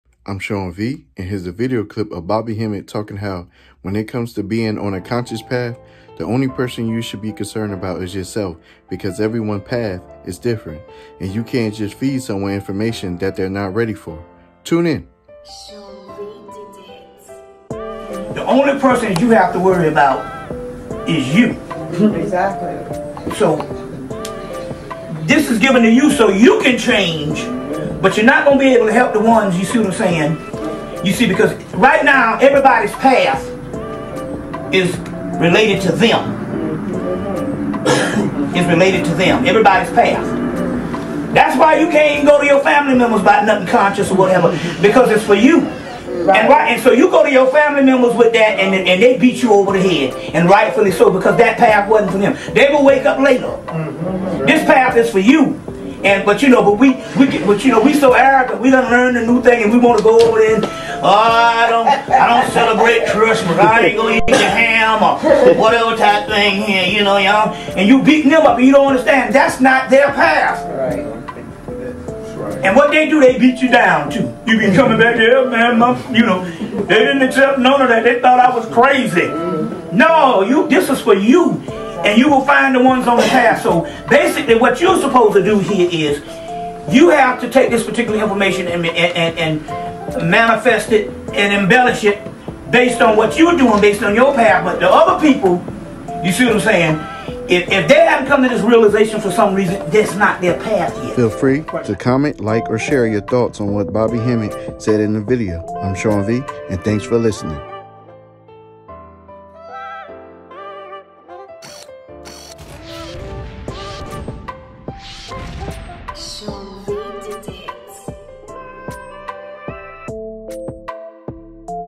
Bobby Hemmitt: Prioritizing Self-Growth and Respecting Individual Journeys
Bobby Hemmitt talking how, when it comes to being on a conscious path, the only person you should be concerned about is yourself because everyone's path is different and you can't just feed someone information that they're not ready for.